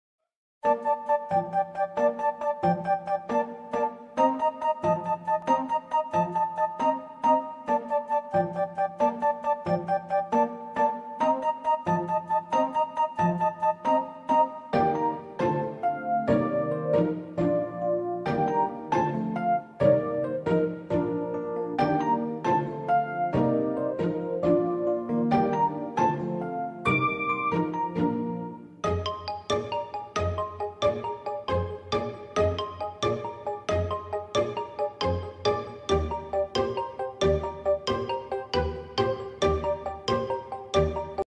Copertura trasparente con cerniere 183X60X165 cm Tatkraft
In vendita qui
Fatto in plastica di buona qualità, questa copertura trasparente è ottima per proteggere non solo gli stand ma anche armadietti esterni.
E' grande 183X60X165cm, le cuciture sono fatte abbastanza bene.
Presenti sono le cerniere che consentono di aprire la copertura senza doverla togliere.
Personalmente ho coperto l'armadietto fuori al terrazzo. Finalmente è a sicuro da pioggia, spruzzi e sporcizia.
Lo consiglio sicuramente.